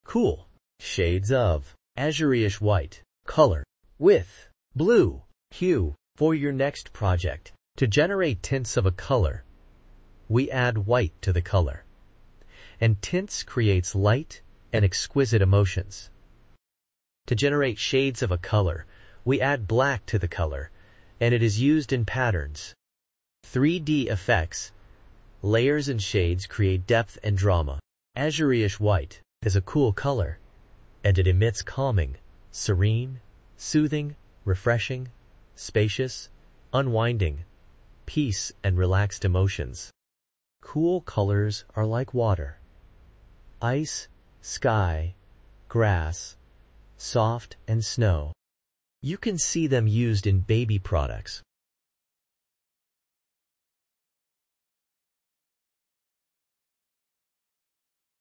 Shades & Tints of Azureish White color dbe9f4 A Cool Blue color
Shades of Azureish White color dbe9f4 Cool Shades of Azureish White color with Blue hue for your project!

Shades
Hex c2cfd8

Tints
Hex dfebf5

Azureish White is a cool color, and it emits calming, serene, soothing, refreshing, spacious, unwinding, peace and relaxed emotions. Cool colors are like water, ice, sky, grass, soft and snow. You can see them used in baby products.